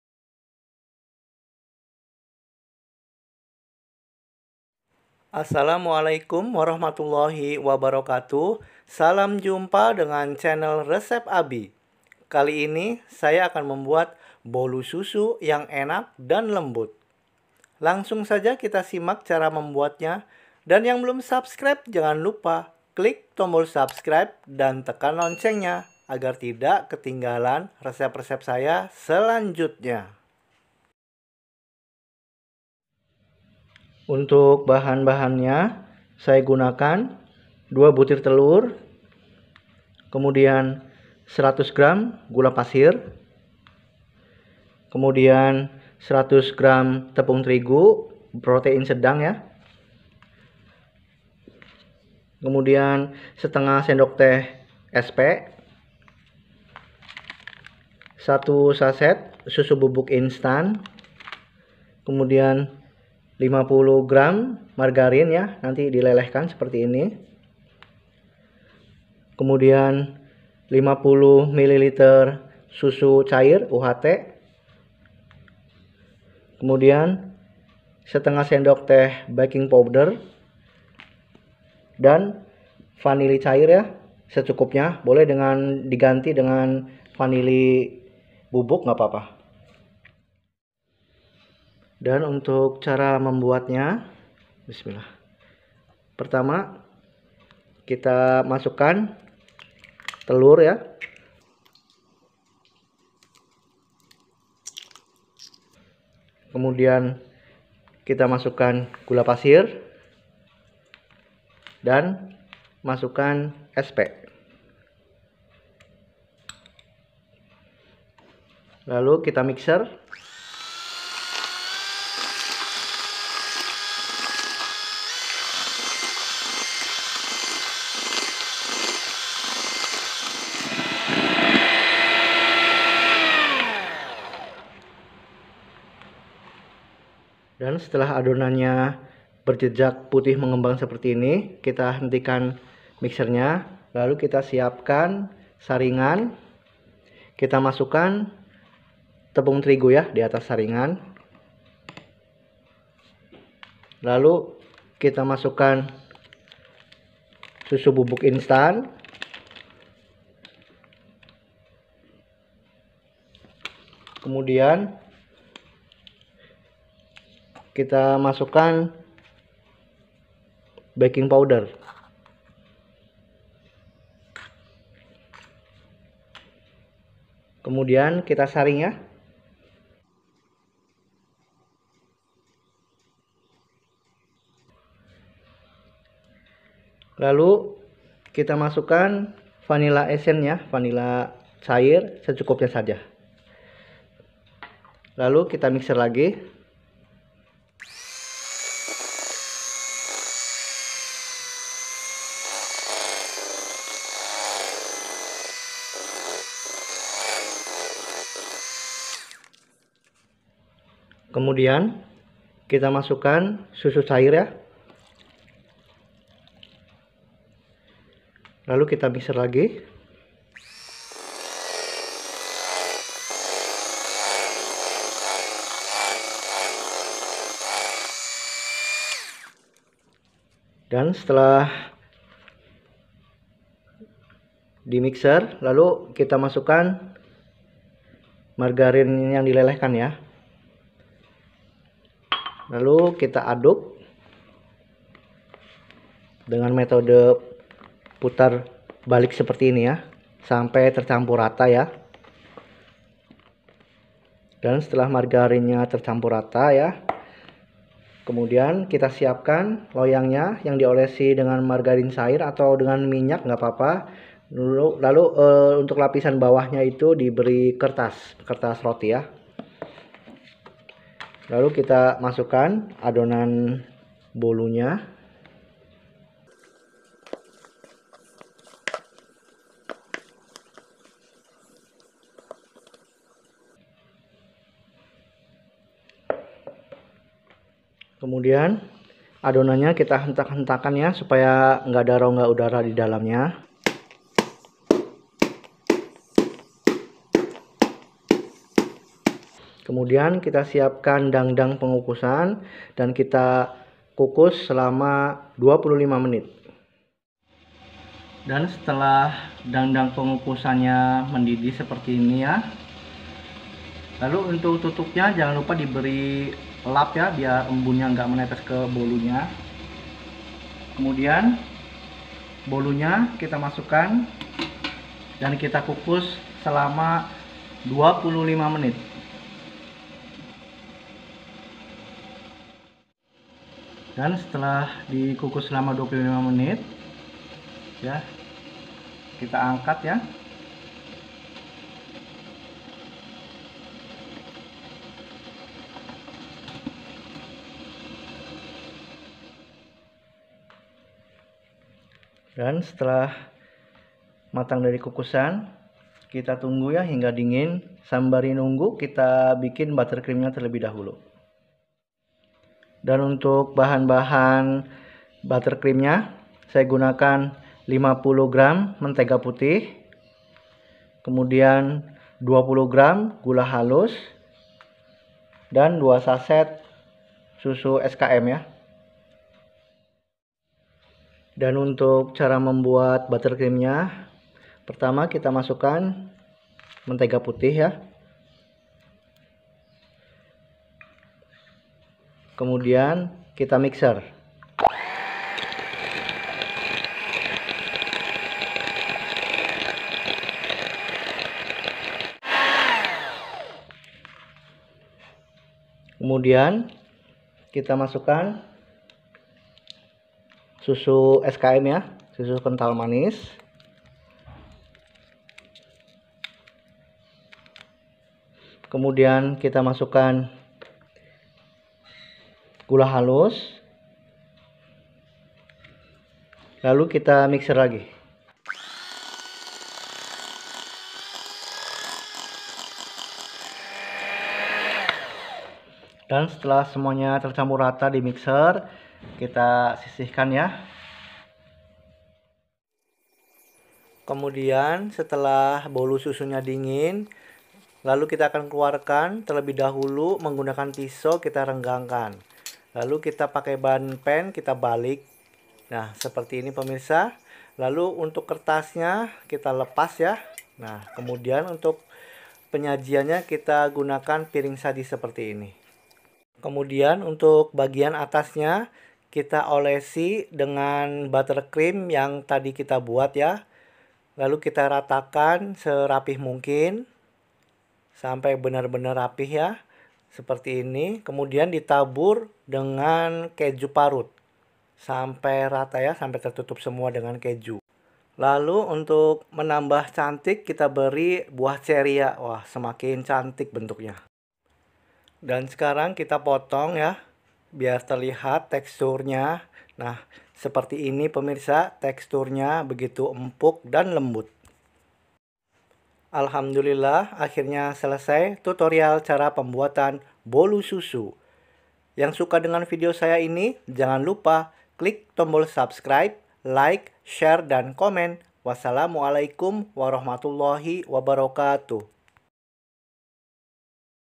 Bolu Susu Cuma 2 Telur Tanpa Oven Super Enak Dan Lembut
For Businees / Endorse
Via DM Instagram

Assalamu'alaikum, jumpa lagi dengan channel resepabi kali ini saya akan membuat Bolu Susu cuma 2 telur yang super enak dan lembut bisa untuk snack box atau untuk ide jualan, berikut ini bahan-bahannya
2 butir telur
100 gr gula pasir
100 gr terigu
1/2 sdt SP
1 sachet susu bubuk
50 gr margarin
50 ml susu cair uht
1/2 sdt baking powder
Vanili secukupnya
Toping keju parut
Untuk cara membuatnya silahkan simak videonya sampai selesai.

jangan lupa tonton vidio nya sampai selesai agar nanti pada saat anda praktek tidak mengalami kegagalan.

Wassalamualaikum.